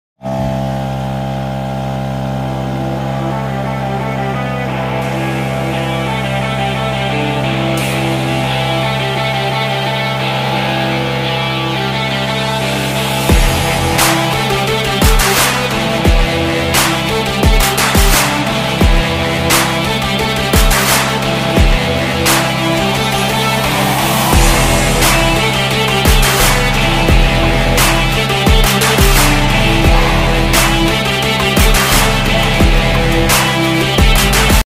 can I?
using my 10ft tender as a rescue tug.